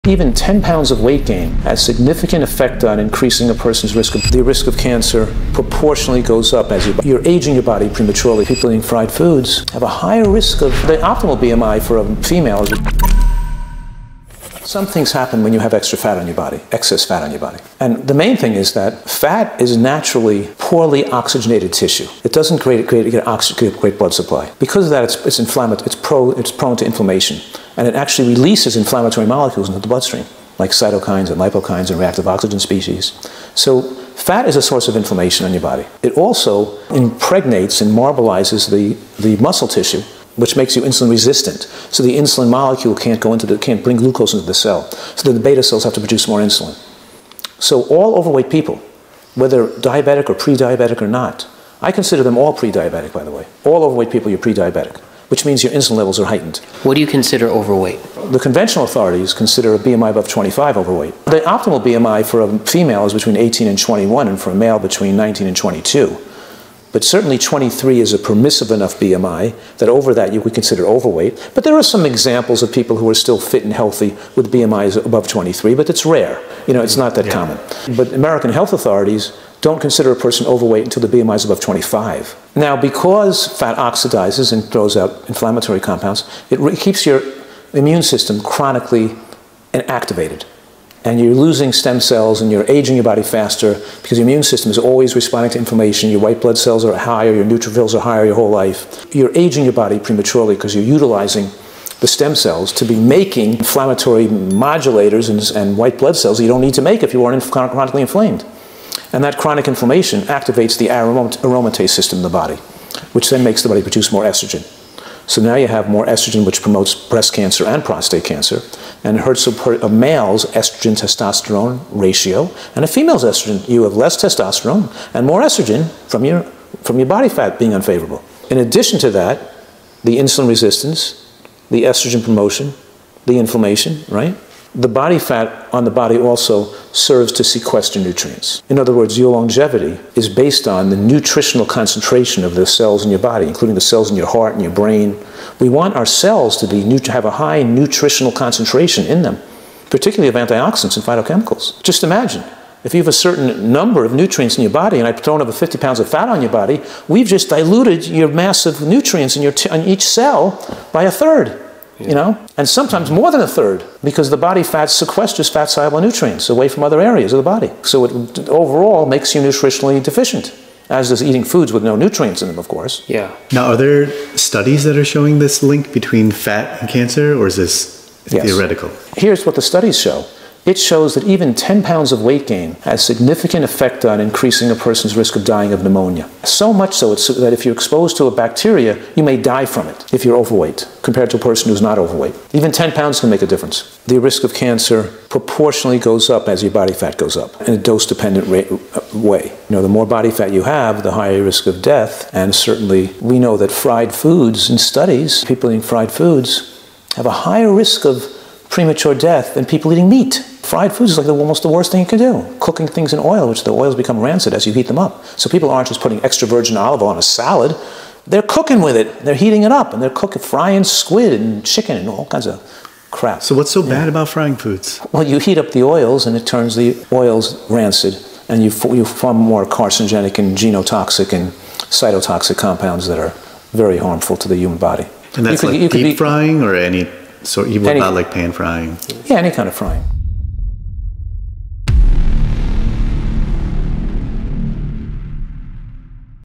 How body fat leads to cancer | Joel Fuhrman
Over 70 million adults in the U.S. are obese (35 million men and 35 million women). 99 million are overweight (45 million women and 54 million men). NHANES 2016 statistics showed that about 39.6% of American adults were obese. Men had an age-adjusted rate of 37.9% and Women had an age-adjusted rate of 41.1%.

Today we have again the fantastic  @drfuhrman who for over 25 years, has shown that it is possible to achieve sustainable weight loss and reverse heart disease, diabetes, and many other diseases by avoiding certain foods and following some principles he has researched over the years.

This video is very critical for both men and women who are concerned about their health and looking to stay cancer-free.

00:00 Intro
00:15 What happens with excess fat on the body
00:57 The perfect BMI for men and women
01:40 Aging body prematurely
02:57 How fat is linked to cancer
03:55 Outro